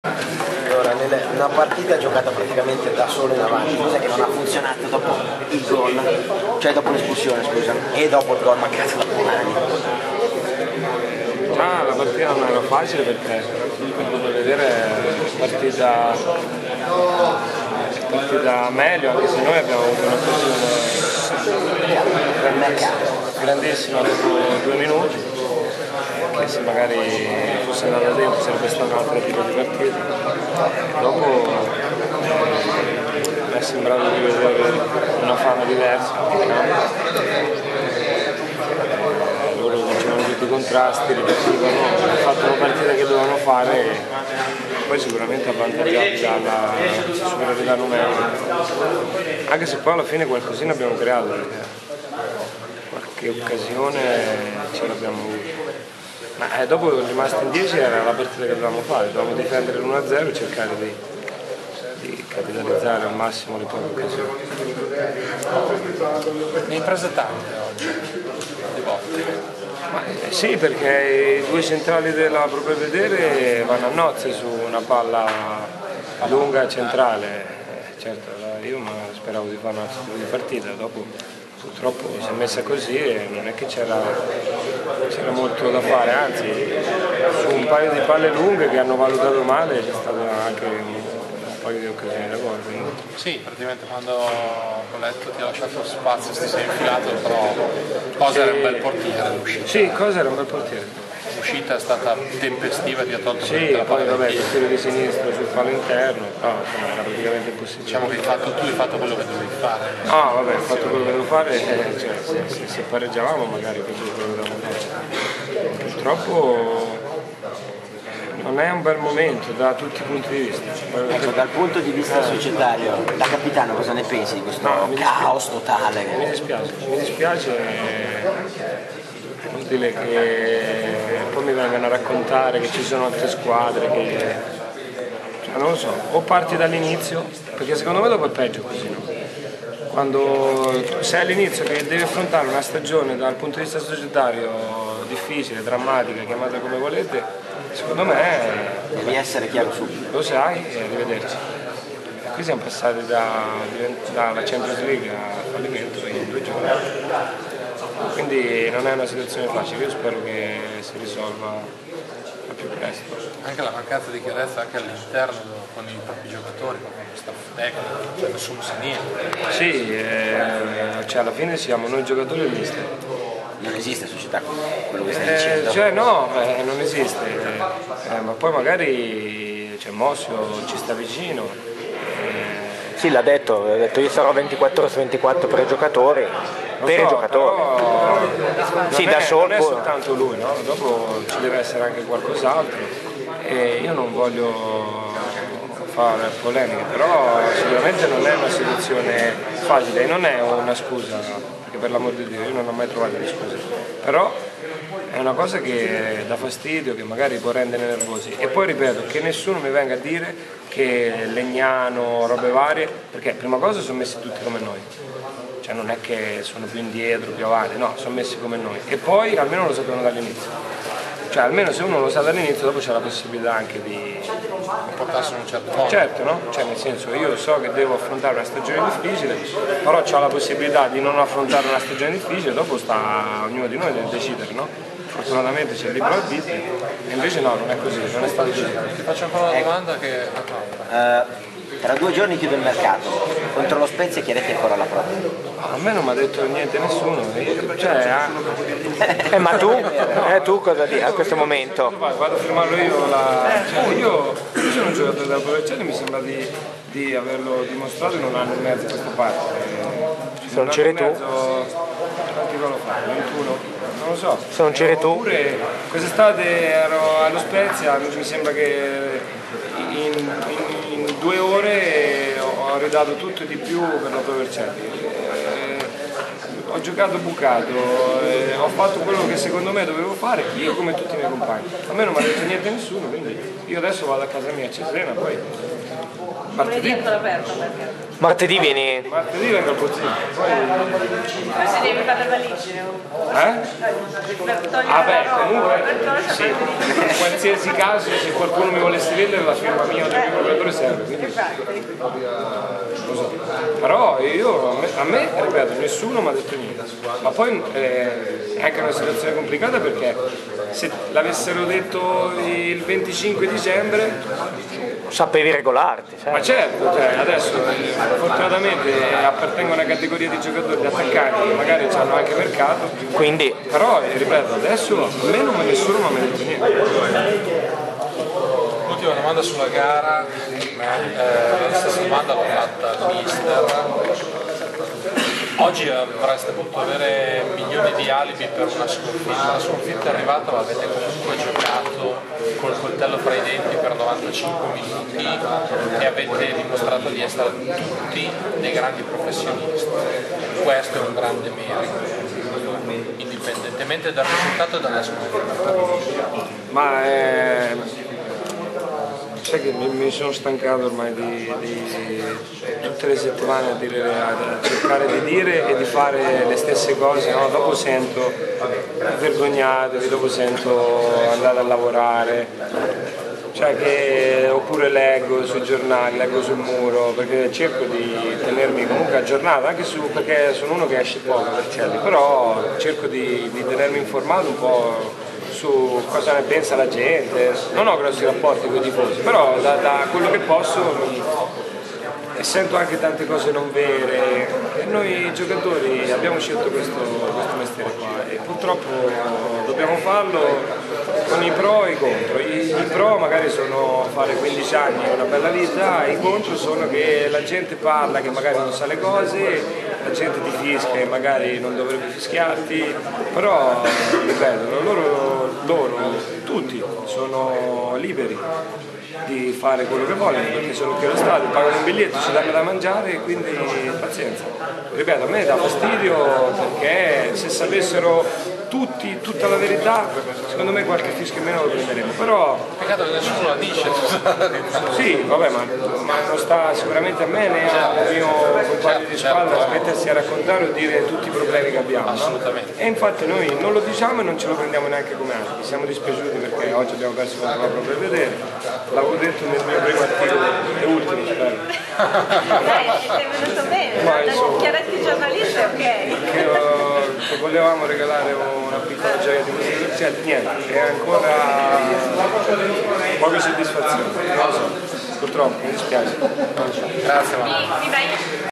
02 Pro Belvedere-Pro Vercelli 1-0 campionato 2009/10
L'attaccante della Pro Vercelli Emanuele Chiaretti intervistato in sala stampa dai giornalisti dopo il derby di ritorno con la Pro Belvedere